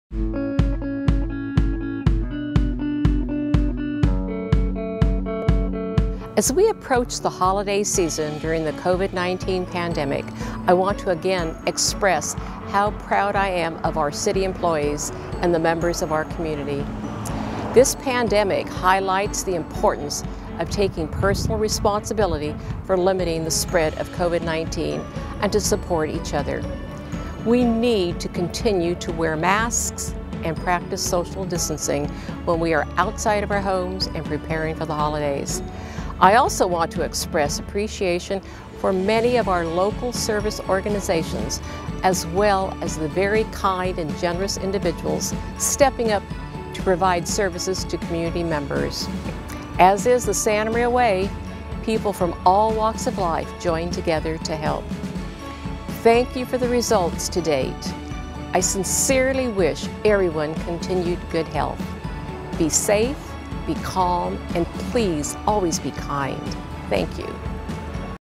Mayor Patino's Holiday Message
As Santa Maria enters the holiday season, Mayor Alice Patino's thanks the community for stepping up to slow the pandemic.